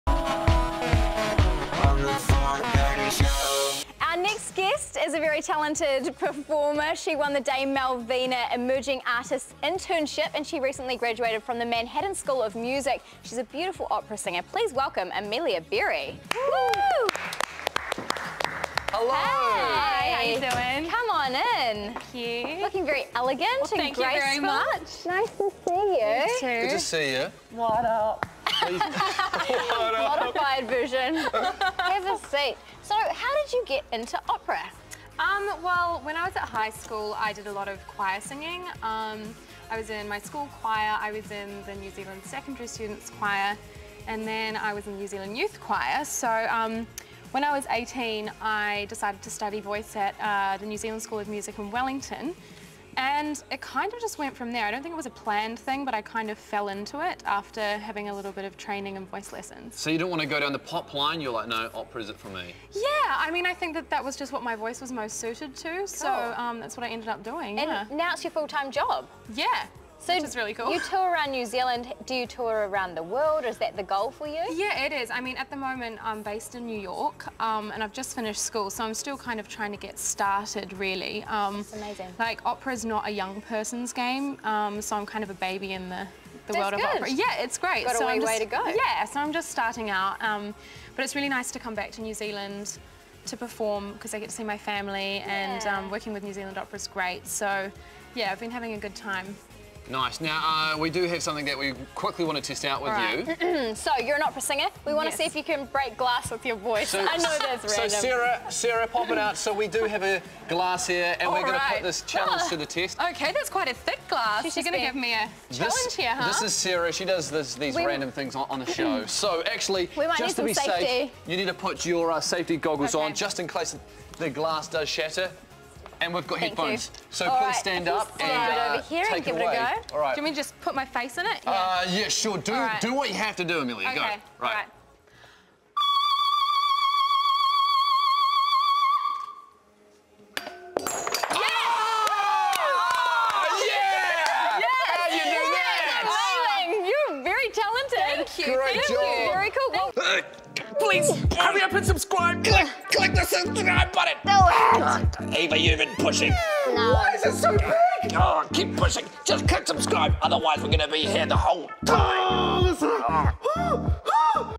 Opera Singer Breaks Wine Glass
Opera singer Amelia Berry manages to break a wine glass after singing a very high note.